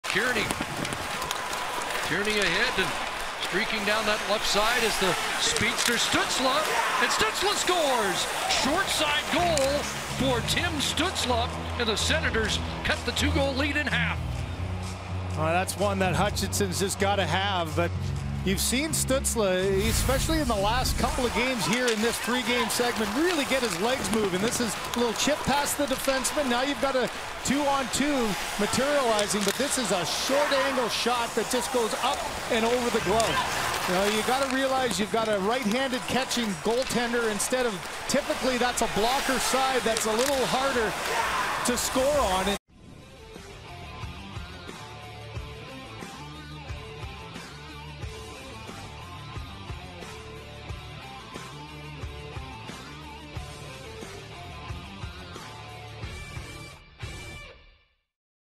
Tim Stutzle Gets Puck By Michael Hutchinson With Fantastic Shot From Tight Angle
Watch as Tim Stutzle makes a nice shot from a tight angle to get the Ottawa Senators on the board.